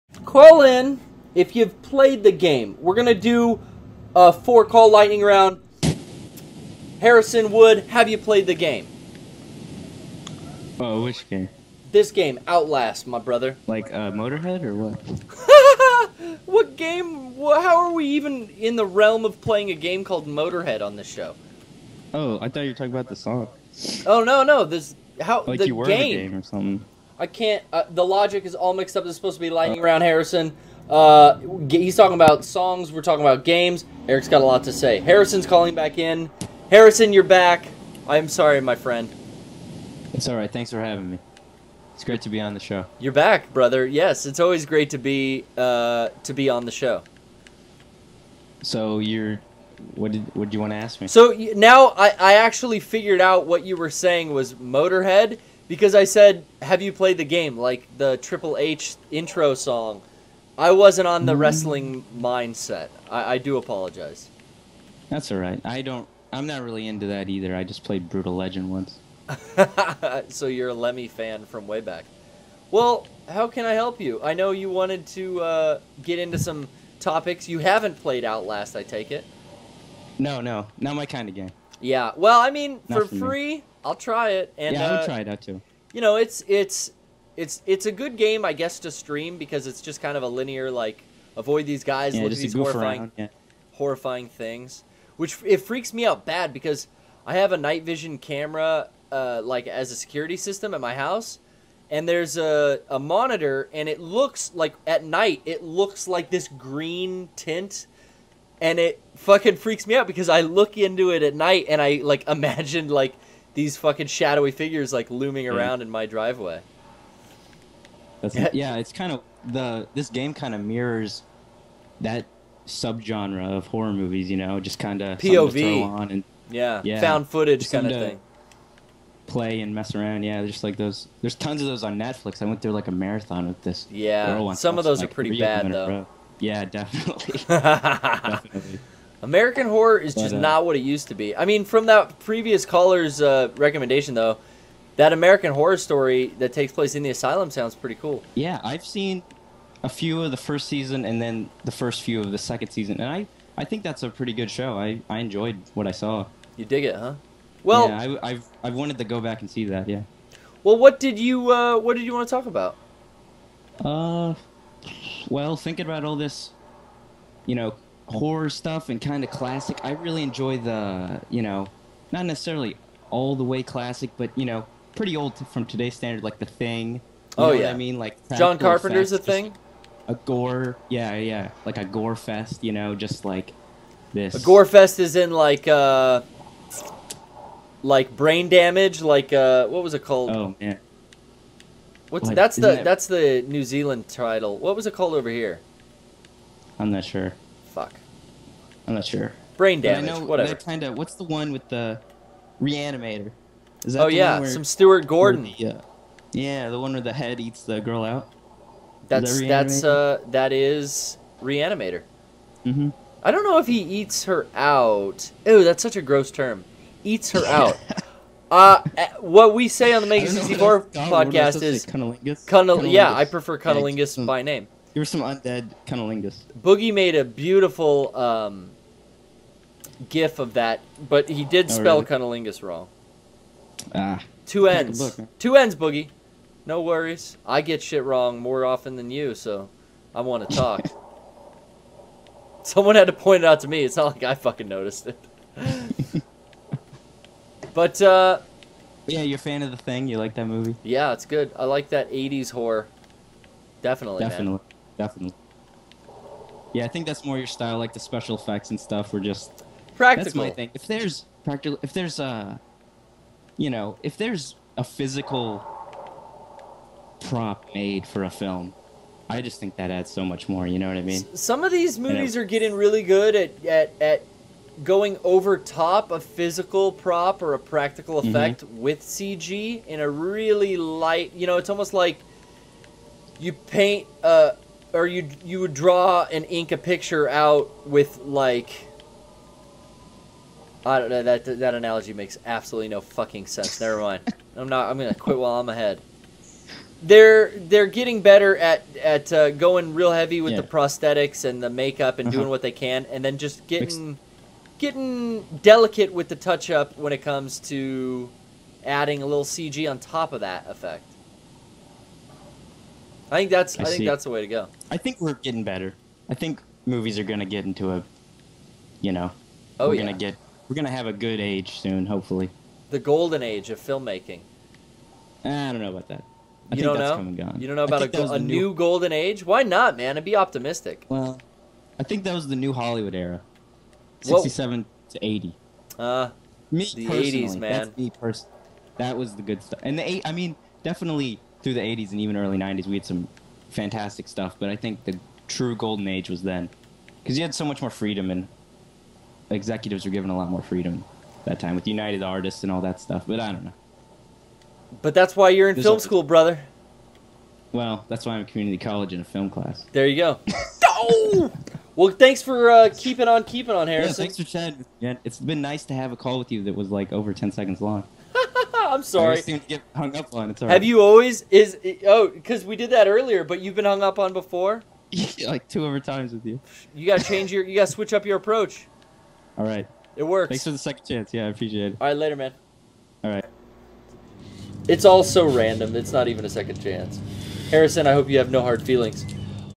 Mega64 Poorly Played Stream 78 - Harrison W.'s Call, Horror Genre
Harrison calls in to discuss a number of horror movies with Garrett.